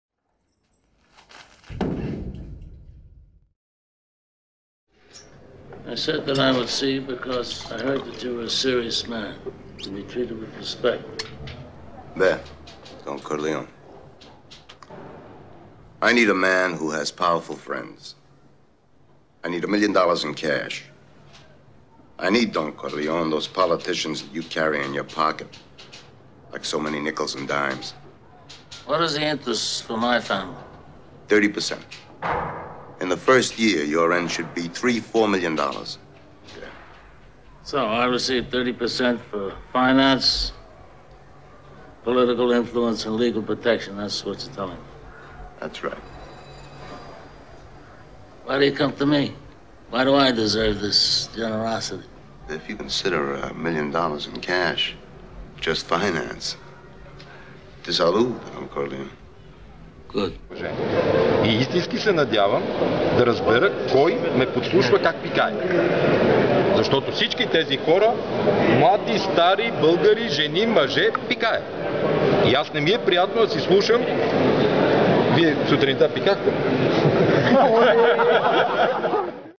РАЗГОВОР БОРИСОВ - ПЪРВАНОВ (СДЕЛКАТА?)
Какво са "договаряли" Борисов и Първанов? - видеокарикатура за сделката според тиражирана информация в някои медии.